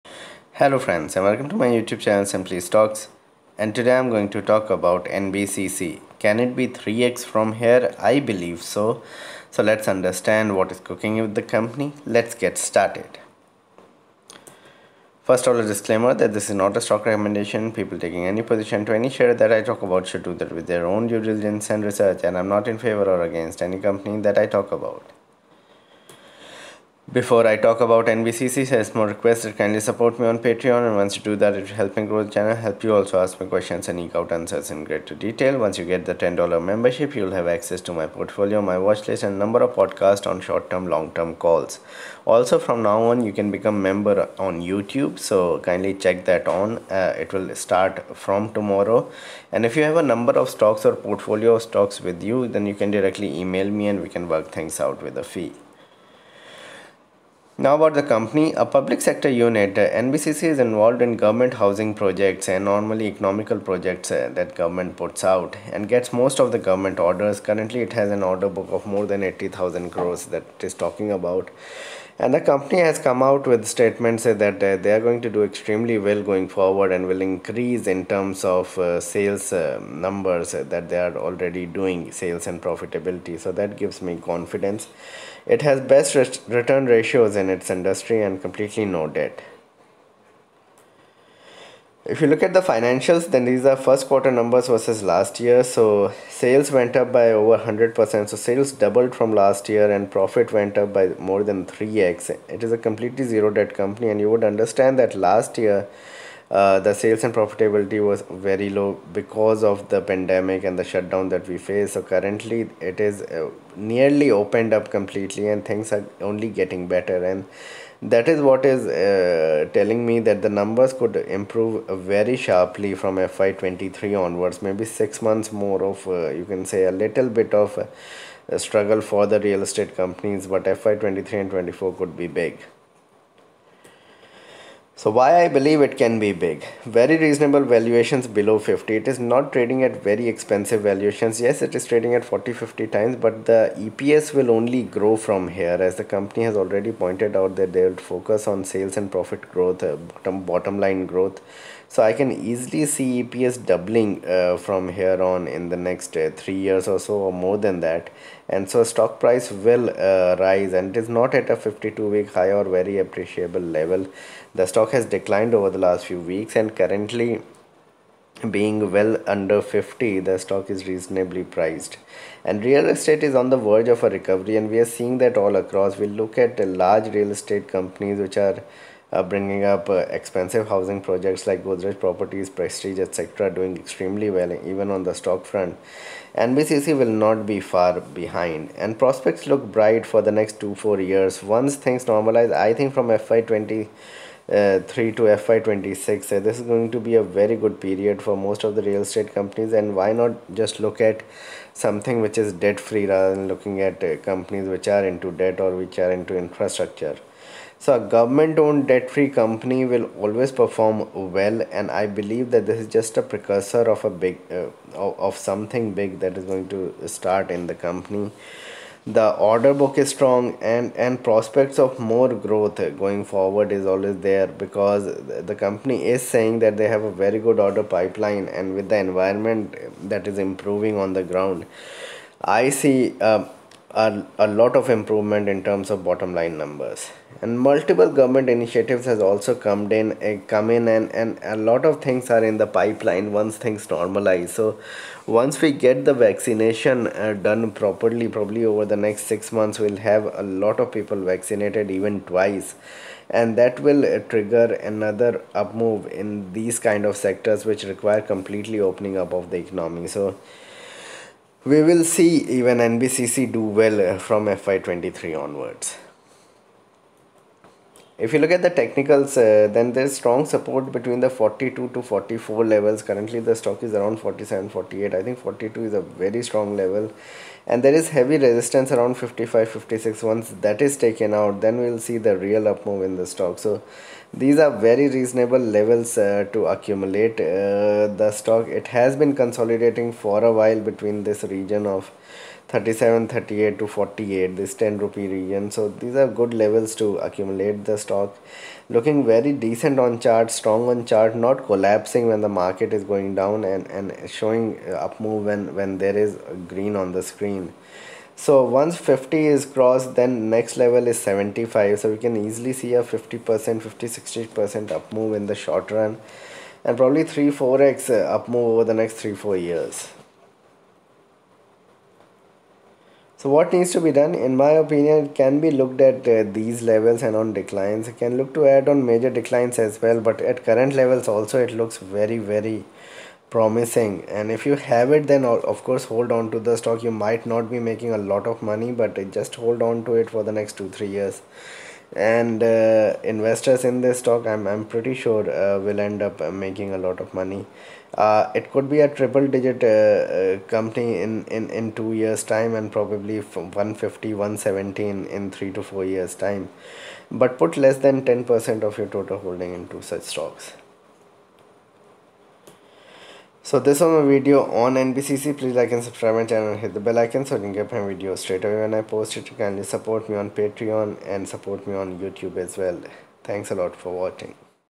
NBCC has been in a 10 point range for many weeks but i feel it can be 3x from here in few years
I analyze nbcc numbers